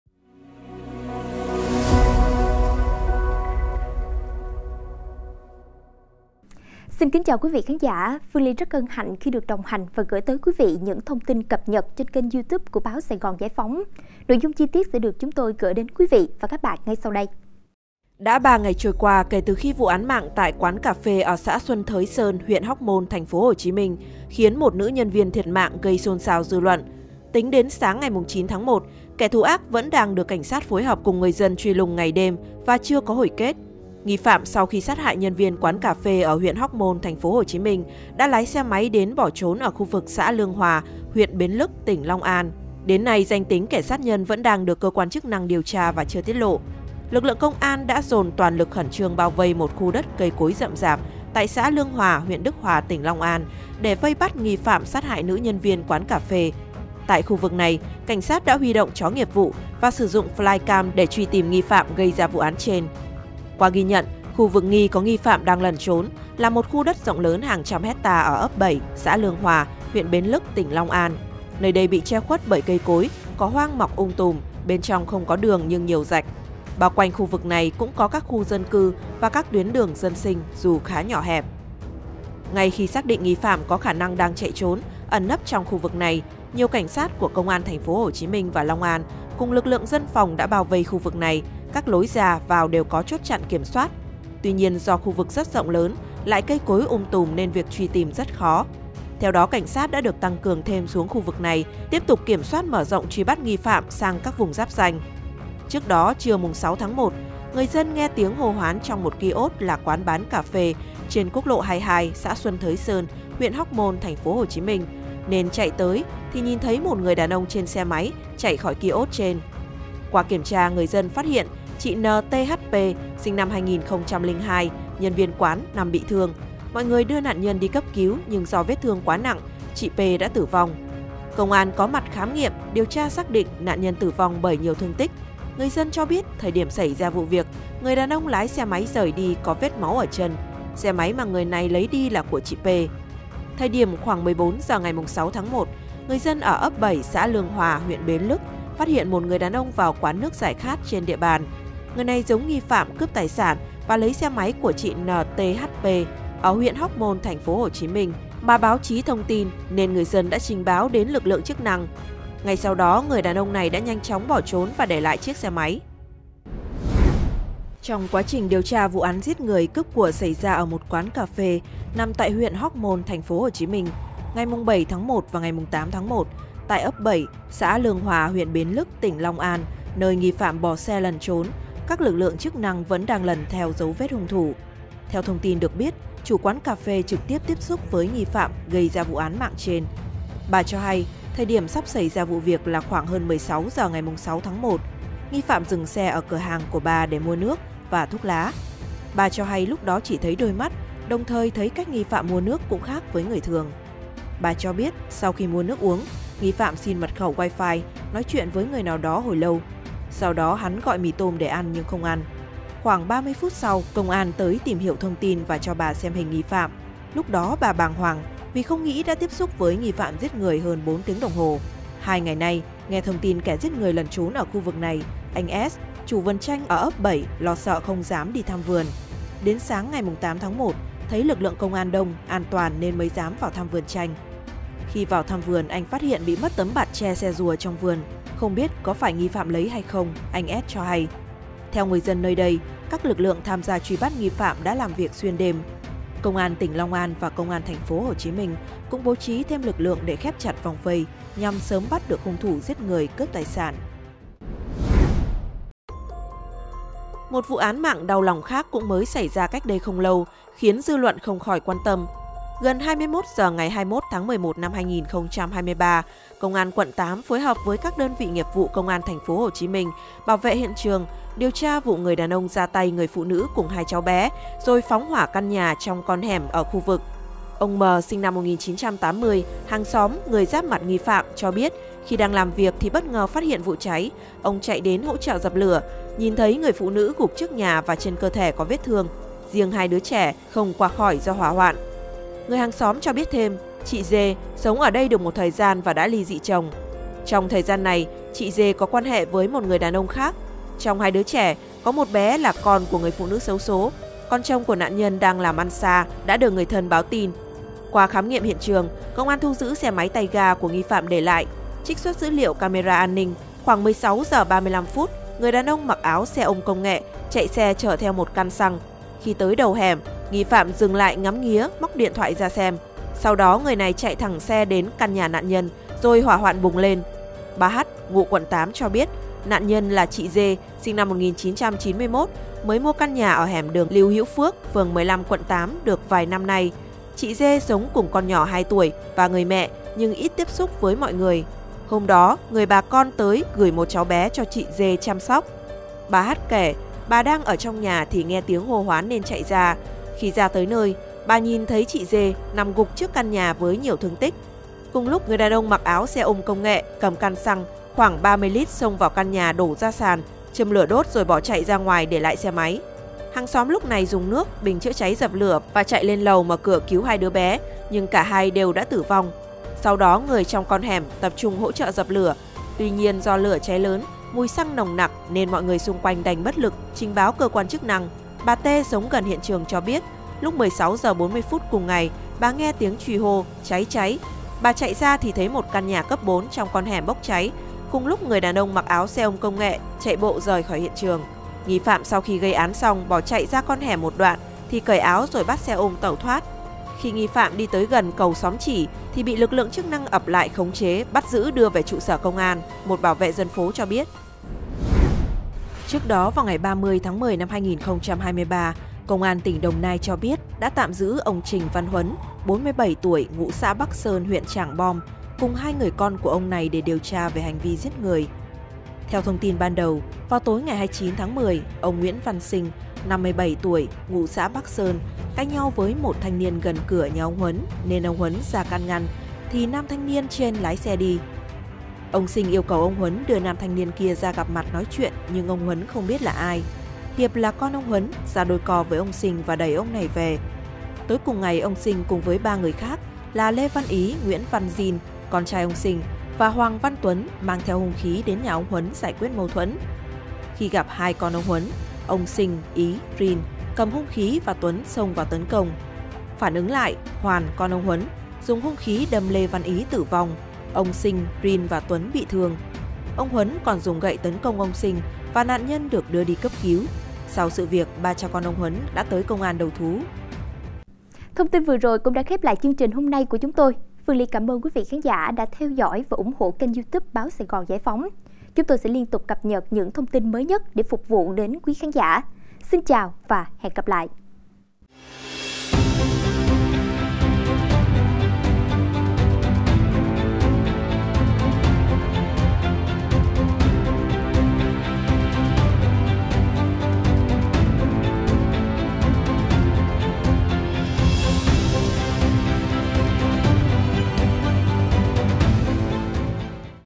Hành trình 3 ngày cam go công an lần theo dấu vết nghi phạm giết người ở Hóc Môn | Tin tức
SGGP | Đã 3 ngày trôi qua kể từ khi vụ án mạng tại quán cà phê ở xã Xuân Thới Sơn, huyện Hóc Môn (TPHCM) khiến 1 nữ nhân viên thiệt mạng gây xôn xao dư luận, tính đến sáng 9-1 kẻ thủ ác vẫn đang được cảnh sát phối hợp cùng người dân truy lùng ngày đêm và chưa có hồi kết. Nghi phạm sau khi sát hại nhân viên quán cà phê ở huyện Hóc Môn (TPHCM), đã lái xe máy đến bỏ trốn ở khu vực xã Lương Hòa, huyện Bến Lức (tỉnh Long An). Đến nay, danh tính kẻ sát nhân vẫn đang được cơ quan chức năng điều tra và chưa tiết lộ.
Lực lượng công an đã dồn toàn lực khẩn trương bao vây một khu đất cây cối rậm rạp tại xã Lương Hòa (huyện Đức Hòa, tỉnh Long An) để vây bắt nghi phạm sát hại nữ nhân viên quán cà phê. Tại khu vực này, cảnh sát đã huy động chó nghiệp vụ và sử dụng flycam để truy tìm nghi phạm gây ra vụ án trên. Qua ghi nhận, khu vực nghi có nghi phạm đang lẩn trốn là một khu đất rộng lớn hàng trăm ha ở ấp 7, xã Lương Hòa, huyện Bến Lức, tỉnh Long An. Nơi đây bị che khuất bởi cây cối, cỏ hoang mọc um tùm, bên trong không có đường nhưng nhiều rạch. Bao quanh khu vực này cũng có các khu dân cư và các tuyến đường dân sinh, dù khá nhỏ hẹp.
Ngay khi xác định nghi phạm có khả năng đang chạy trốn, ẩn nấp trong khu vực này, nhiều cảnh sát của Công an TPHCM và Long An cùng lực lượng dân phòng đã bao vây khu vực này, các lối ra, vào đều có chốt chặn kiểm soát. Tuy nhiên, do khu vực rất rộng lớn, lại cây cối um tùm nên việc truy tìm rất khó. Theo đó, Cảnh sát đã được tăng cường thêm xuống khu vực này tiếp tục kiểm soát mở rộng truy bắt nghi phạm sang các vùng giáp ranh.